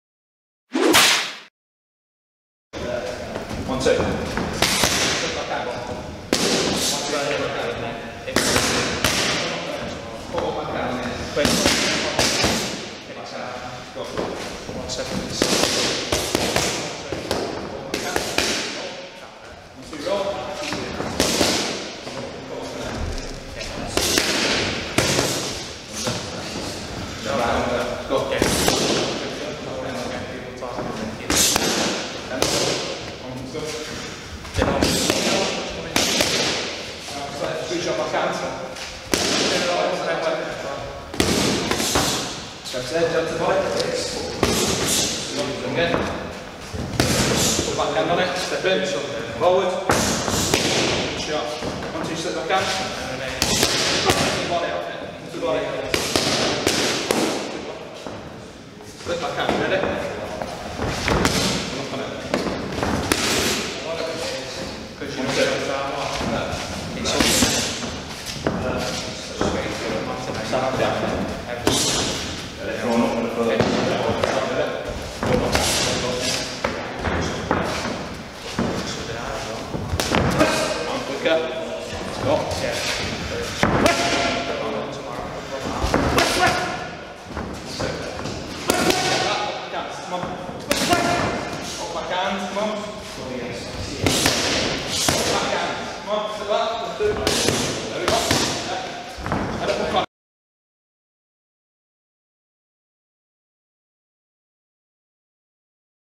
90 Second Boxing Pad Work with coach Lewis Broad
90 Second Boxing Pad Work with coach Lewis Broad of Astley VIP